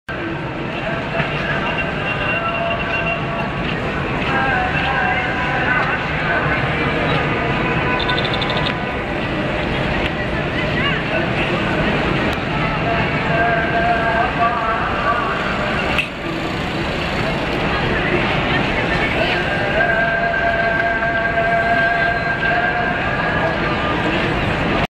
Tahrir Overview 9.35 Raw 2
Tear Gas still being throw intermittently in Simon Bolivar square. Has been going on all night.